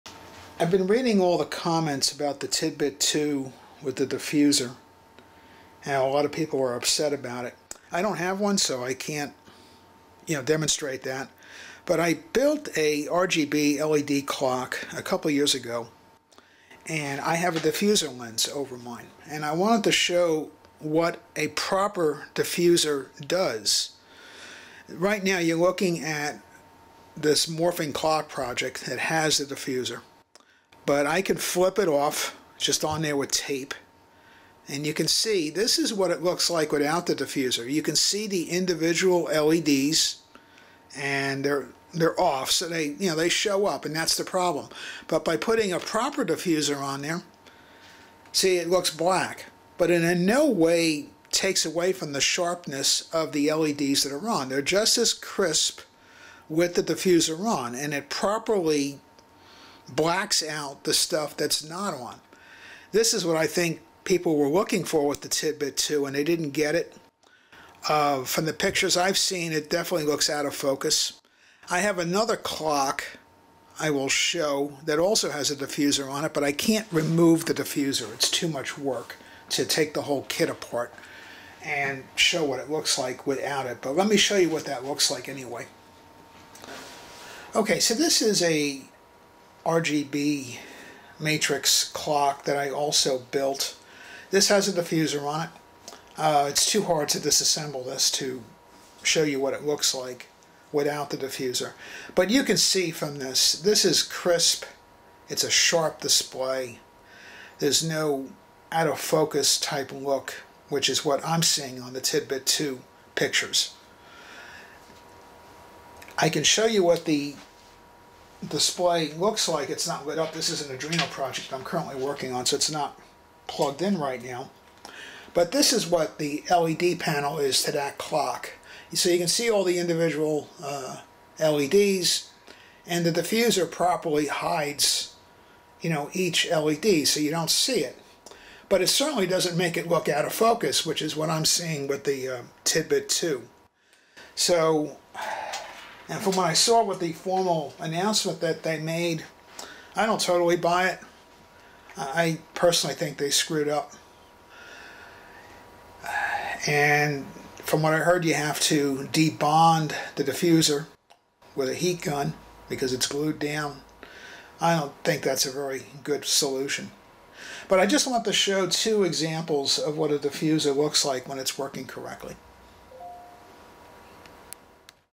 Diffuser examples for LED projects
After reading all the complaints about the Tidbyt 2 diffuser issues I want to show how I think a diffuser should look when it's done properly.

Lystaii 9pcs Gel Light Filter Color Correction Colored Overlays Transparent Color Film Lighting Gel Filter Plastic Sheets Correction 11.7 by 8.3 Inches for Film, Video, Photo, Stage (Tea Black)